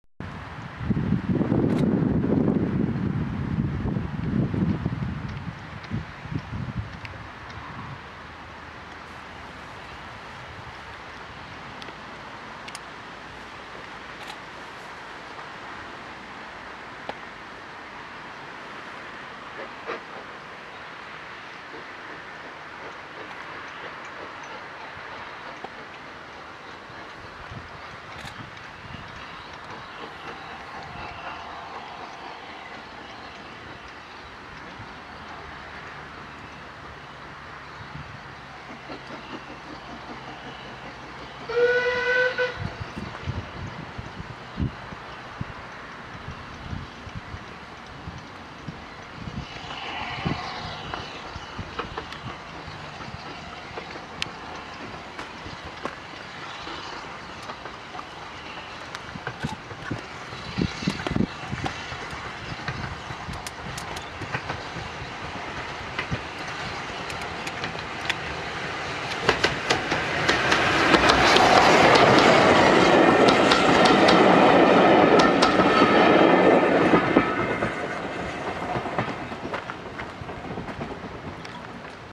swanage railway autumn steam gala 2014 17/10/2014
met tank leaves corfe castle en route to norden with an early morning train swanage railway autumn steam gala 2014